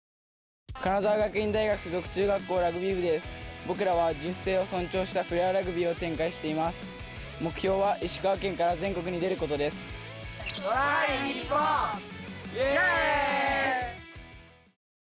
ラグビーW杯2023『トライ！日本！』金沢学院大学附属中学校ラグビー部 編
金沢学院大学附属中学校ラグビー部のみなさんにご出演いただきました！

いよいよラグビーワールドカップ2023開幕！！
みんなで盛り上がりましょう！！

9月8日開幕